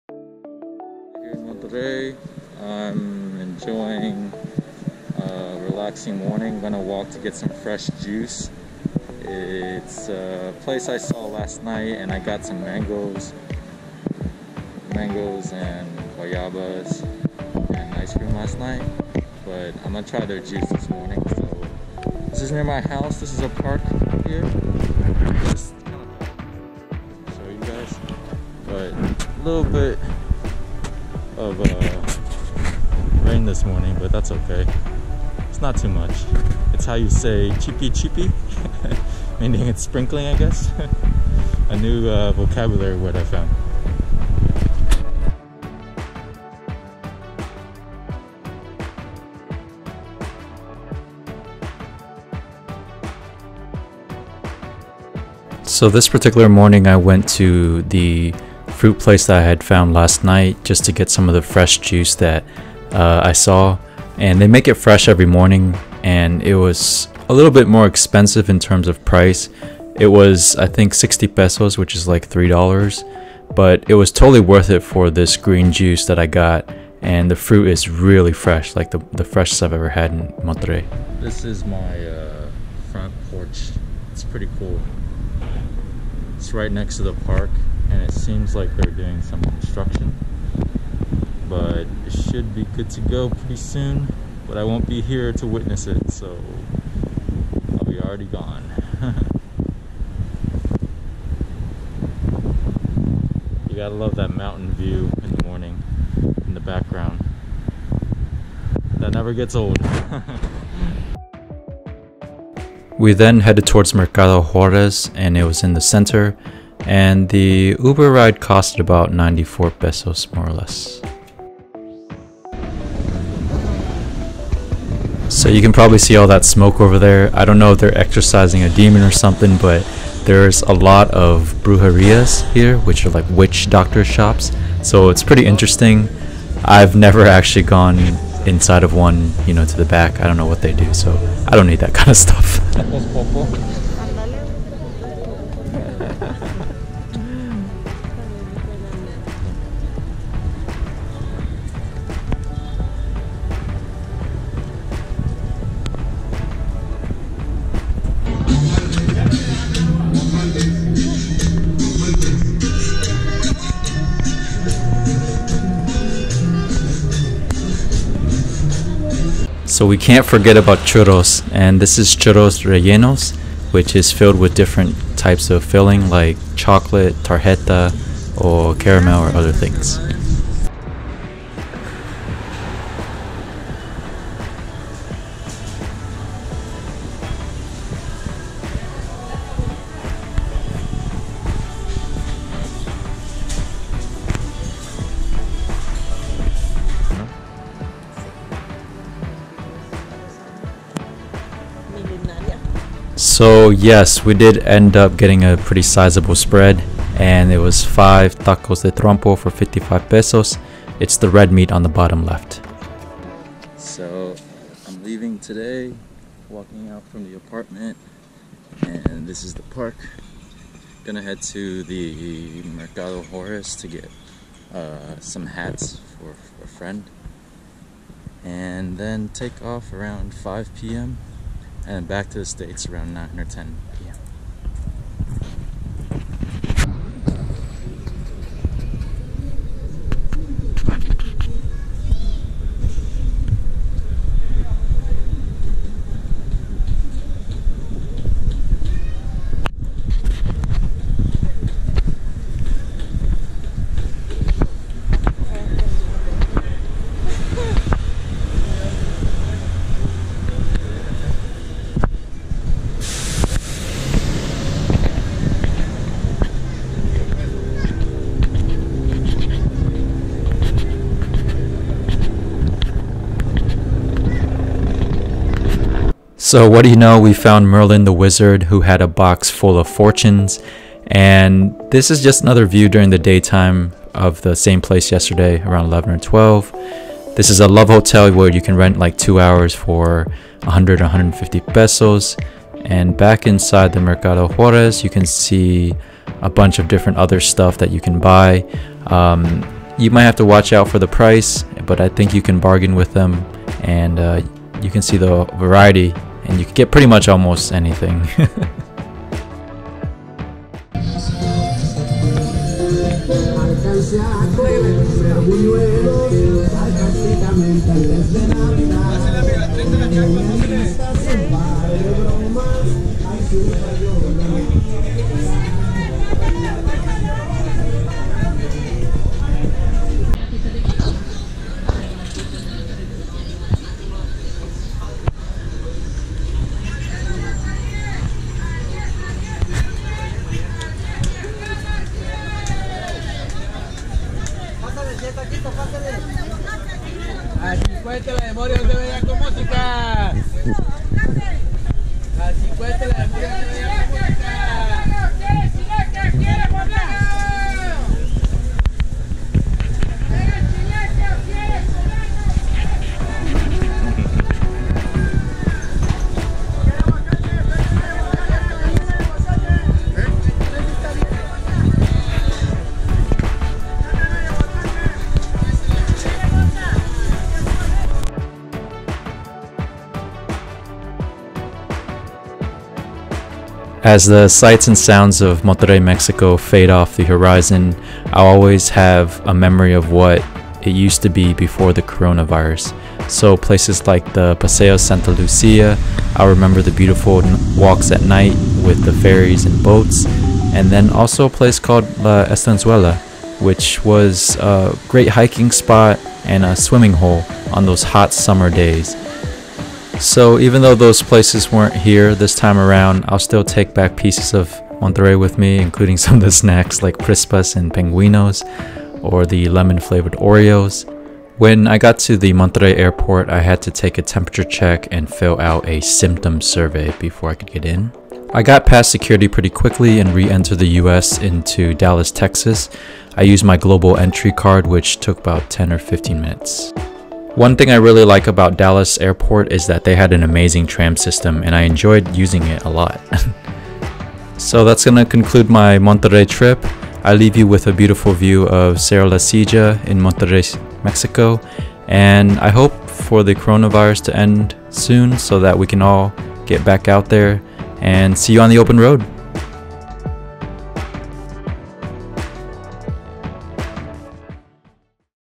San Nicolás de los Garza and Mercado Juarez in Monterrey, MX August 2020 | travelDev Videos (225)
This morning I went to grab some freshly made green juice and hung on my porch in the morning to enjoy the fresh rain air. Later in the day in the afternoon, we went to Mercado Juarez to explore and buy some things. We grabbed some delicious tacos from Popo's and wings from Bontanaz Fokker and had a real feast. On departure day, we stopped again by the Mercado Juarez since it was on the way to the airport. I got through customs and security just fine and re-entered the US by way of Dallas, Texas. Global Entry card is fantastic FYI! I left the video with some of my closing thoughts of my wondering trip this time in Monterrey, Mexico.

Topics
0:01 Heading to the Fruiteria Orange
1:50 Mercado Juarez (Brujeria, Plaza Colegio Civil, Churros)
3:00 Grabbing dinner from Popo's Tacos and Bontanaz Fokker Wings
3:33 Departure day (Buying souvenirs at Mercado Juarez again, Merlin the Wizard)
6:18 Closing thoughts of my Monterrey trip